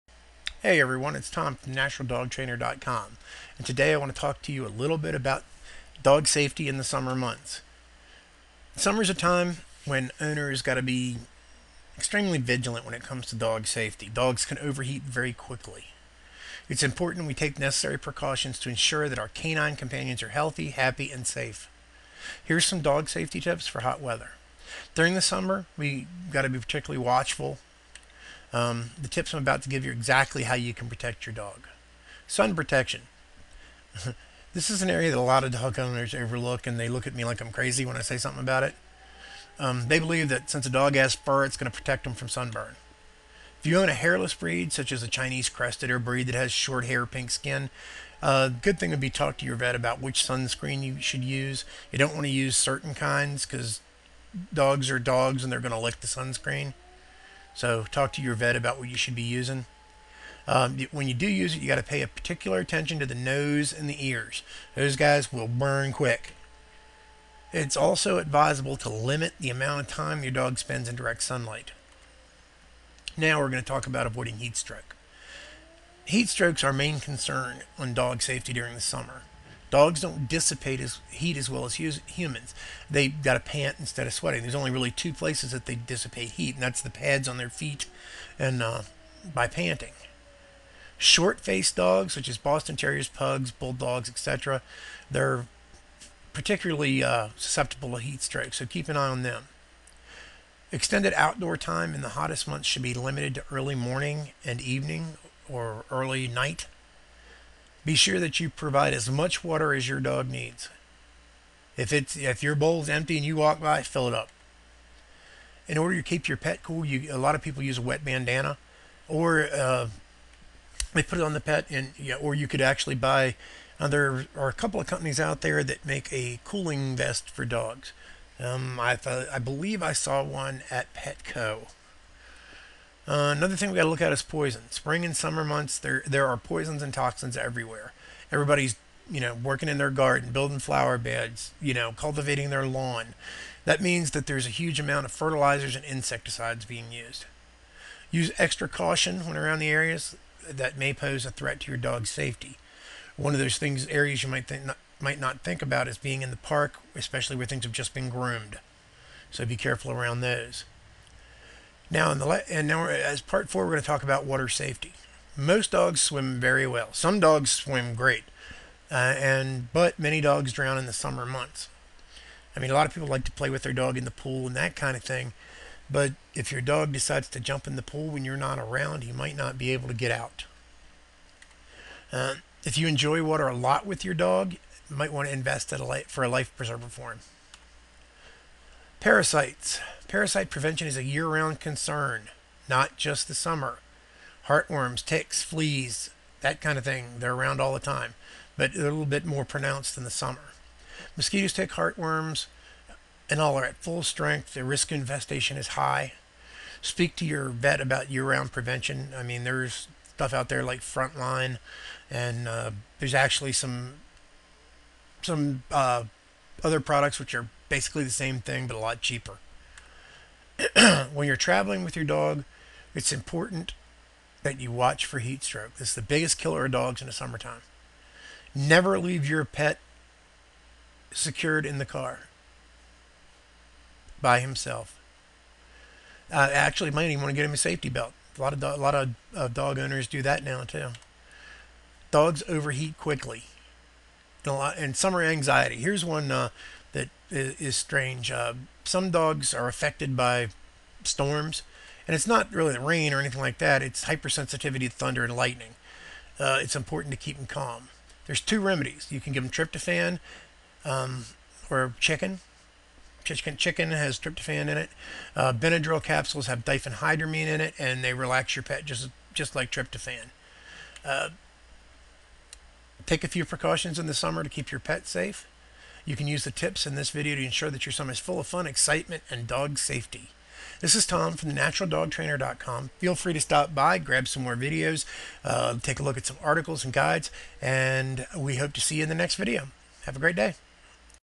Dog Safety in the Summer Months.mp4
Dog safety is a concern for all seasons but particularly in the summer months. Learn how to keep your dog safe during this season.